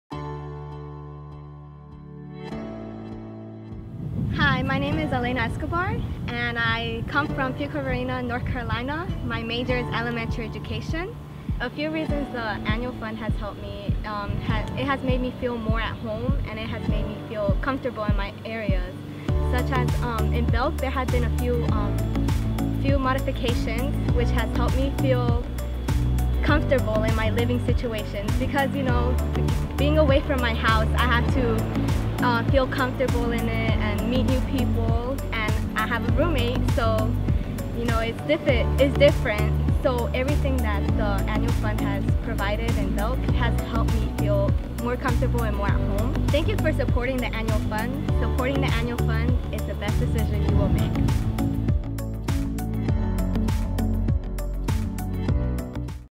Here's How Elena Sees Chowan Moving Forward
Every gift to the Annual Fund supports Chowan’s greatest needs and continues to move Chowan Forward. Learn how your gift moves Chowan forward through the eyes of Elena Escobar.